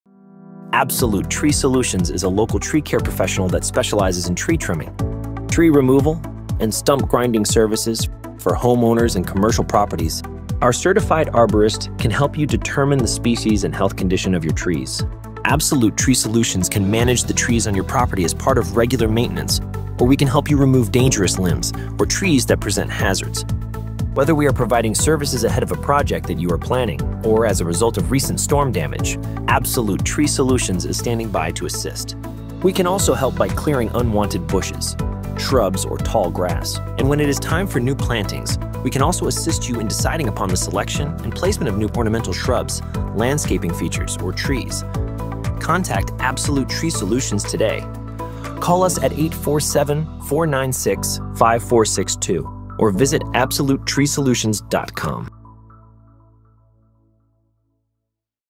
Absolute Tree Solutions | Local Tree Removal and Tree Trimming Contractor
Absolute Tree Solutions is a local Arborist, tree care professional, plus tree trimming and tree removal contractor. We are locally based in Palatine, IL and we serve our surrounding suburbs.